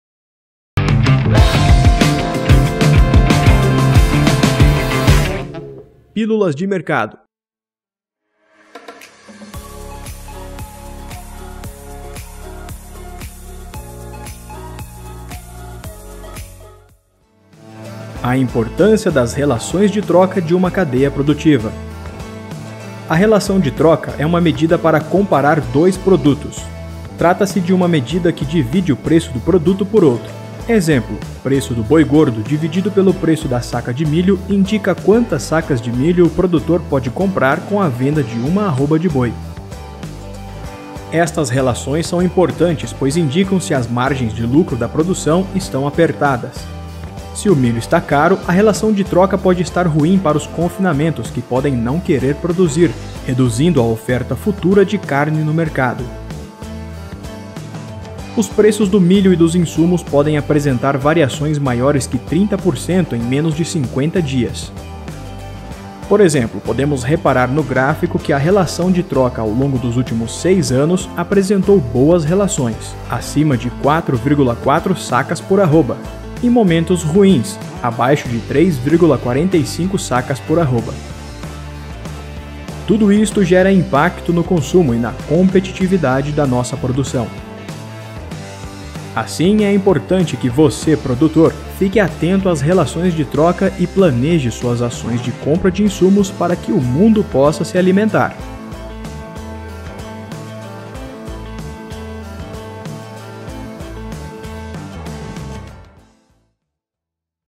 Pílulas de Mercado: A importância das relações de troca de uma cadeira produtiva
A relação de troca é uma medida para comparar dois produtos. Trata-se de uma medida que divide o preço do produto por outro. Relações importantes que indicam se as margens de lucro da produção estão apertadas.